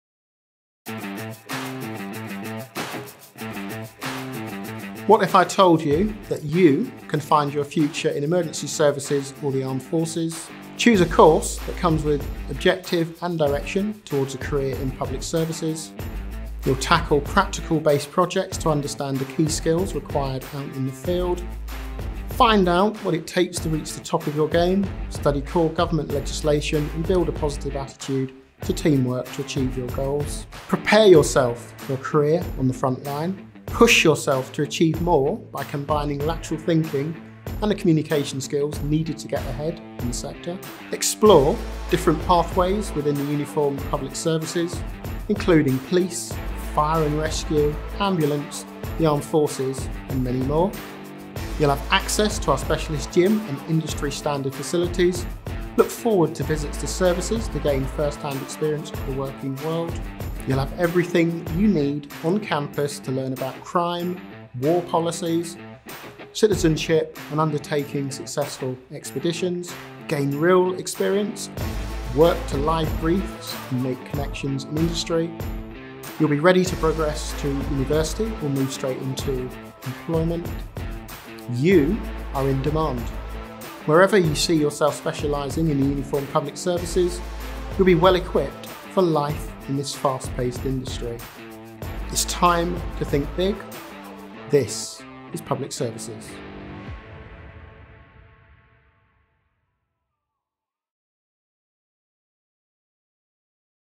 This is Public Services
What if I told you that you can find your future in emergency services or the armed forces. Choose a course that comes with objective and direction towards a career in public services. You’ll tackle practical-based projects to understand the key skills required out in the field. Find out what it takes to reach the top of your game, study core government legislation and build a positive attitude to teamwork to achieve your goals. Apply today and prepare yourself for a career on the front line.